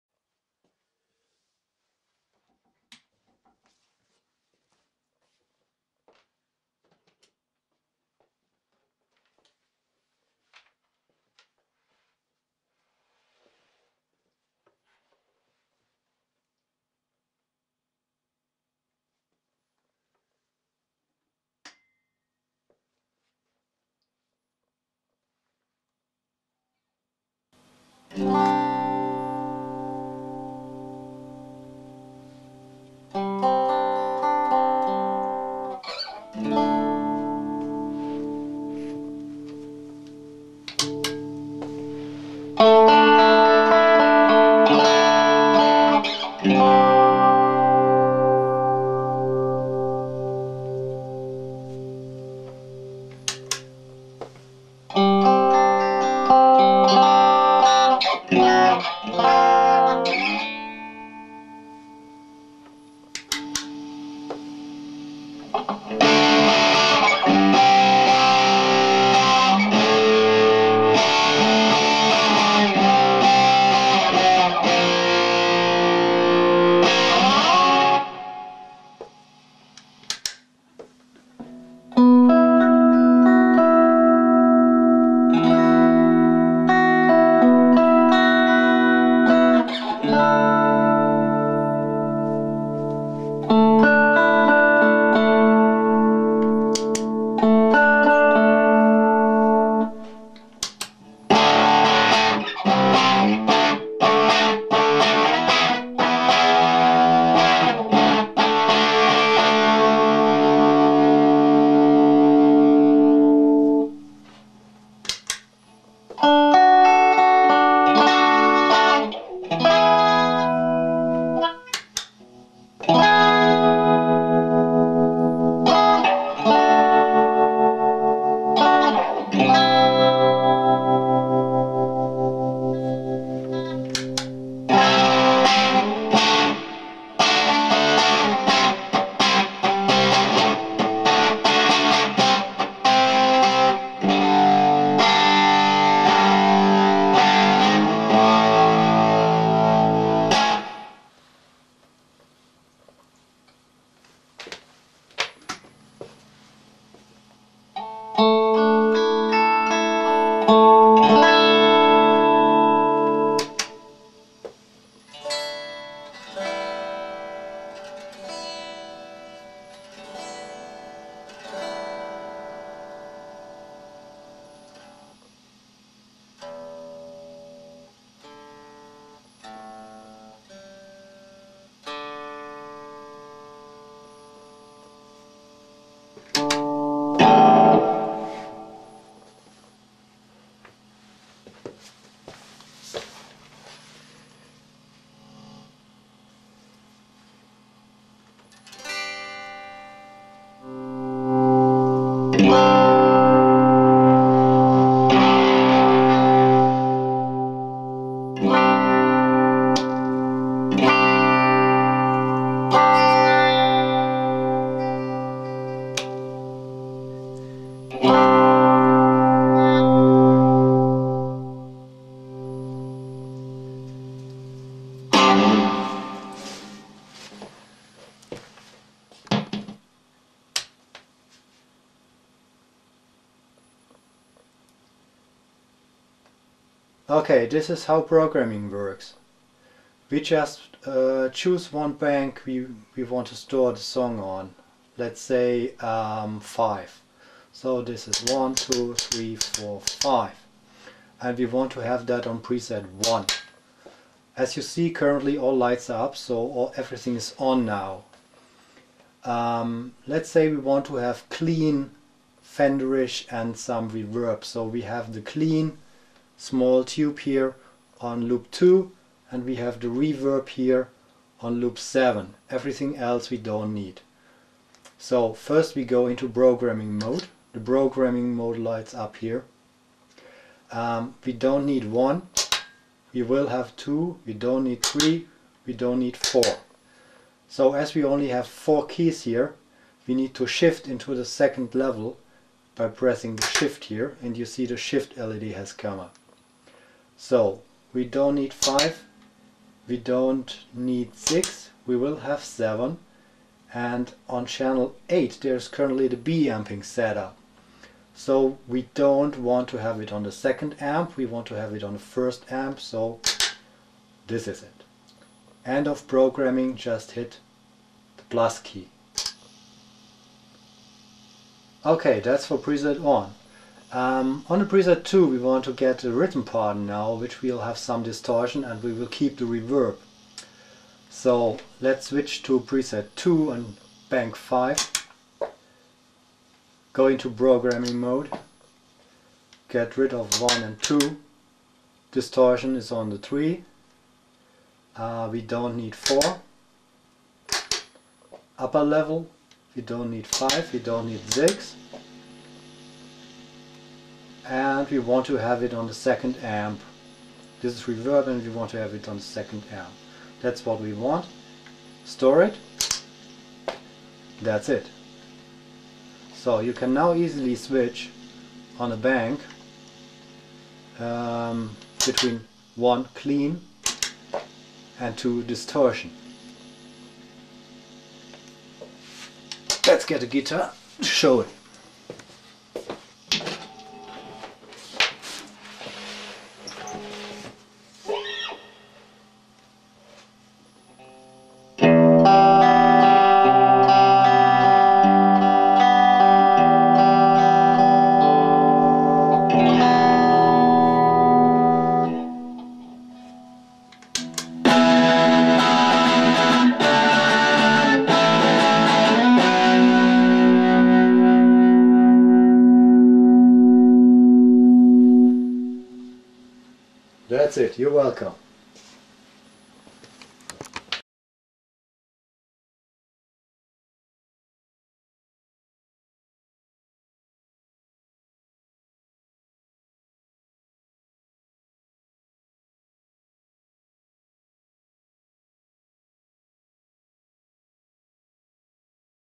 PedalControl 2.0 Demonstration
Demonstration of our DIY Pedal project and how is used.
for more information.